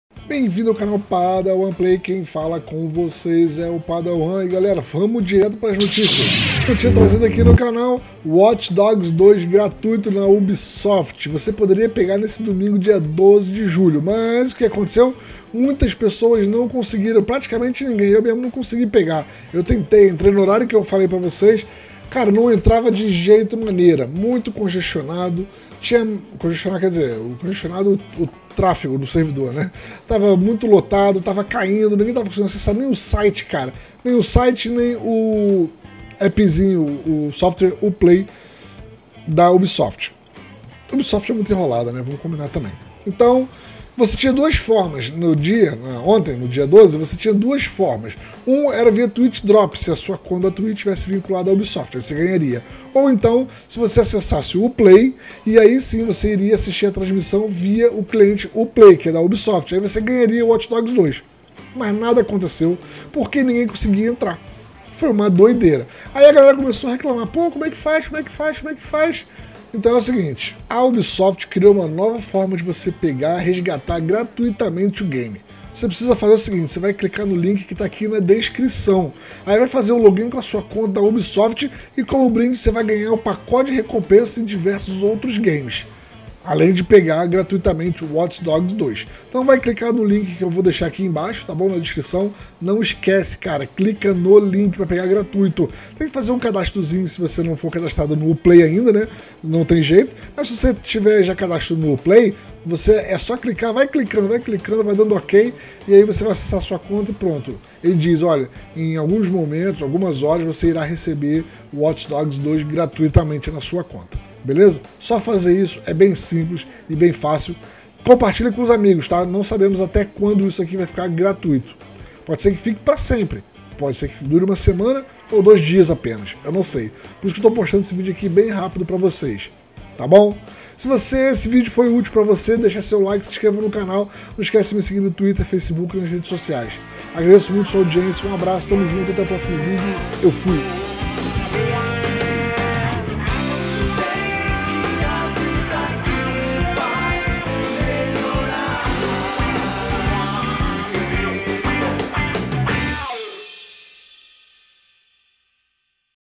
PEGUE AGORA WATCH DOGS 2 DE GRAÇA
Neste vídeo te mostro como baixar Watch dogs 2 de graça.  Mas faz rápido pq pode sair do ar.